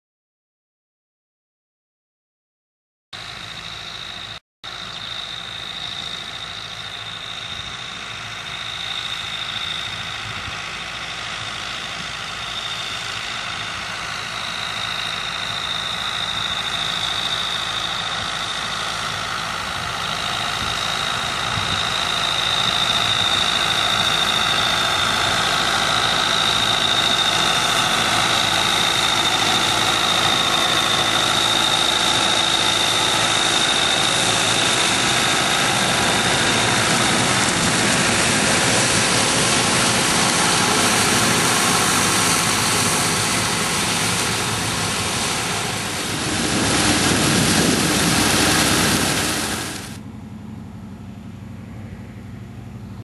SV250SE.wmv
This is a new concept for the treatment of all types of grasses and straw.
These swath wilters will pick-up the material while removing stones, dirt and debris.
This machine will gently turn, aerate and move the hay, grass or straw to one side (reversable) using a draper at thee discharge point, while producing a light, fluffy swath.